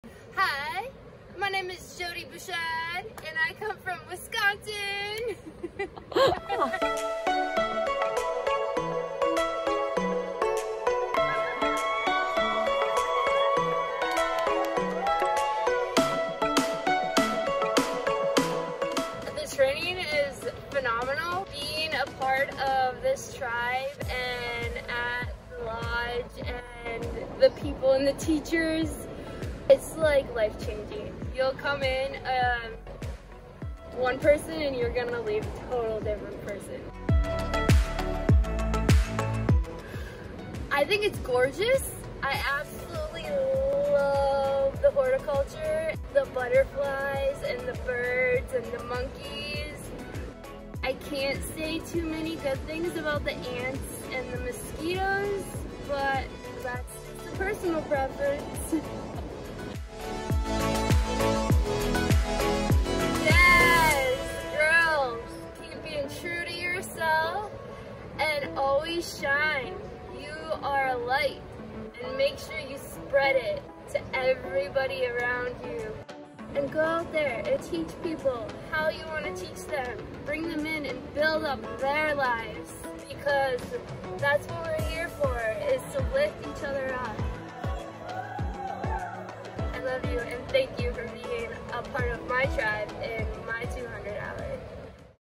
Jodi's Experience
Jodi's review of our Amazon Jungle intensive 200 hour yoga teacher training at Moksha Yoga Amazónica, November 2021